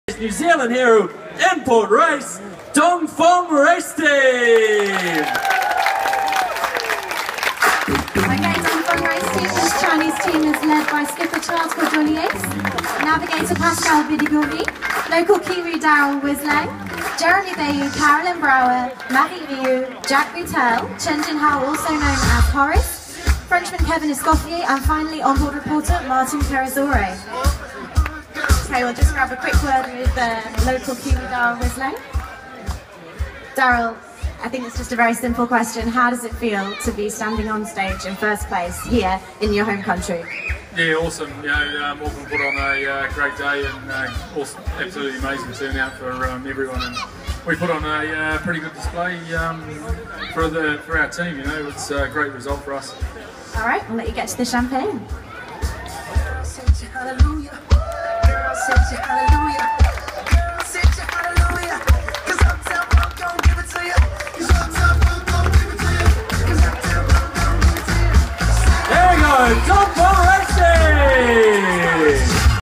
Dongfeng wins Auckland in port race for the VOR
Daryl Wislang (NZ) enjoying the champagne moment with his daughter after Dongfeng won the in port race in Auckland for the 2017/18 Volvo Ocean Race